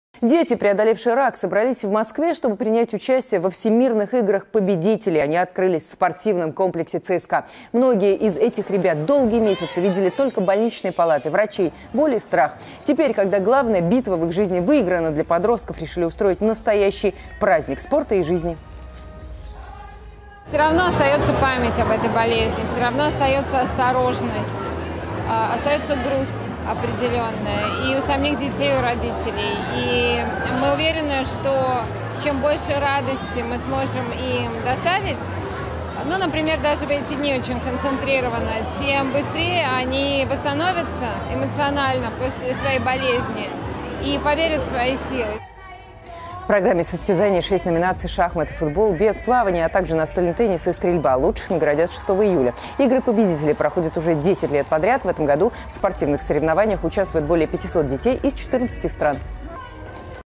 Победившие рак дети выступят на Всемирных играх победителей/ПОДРОБНОСТИ В ОПИСАНИИ
Дети, преодолевшие рак, собрались в Москве, чтобы принять участие во Всемирных играх победителей, они открылись в спортивном комплексе ЦСКА.

Многие из этих ребят долгие месяцы видели только больничные палаты, врачей, боль и страх. Теперь, когда главная битва в их жизни выиграна, для подростков решили устроить настоящий праздник спорта и жизни.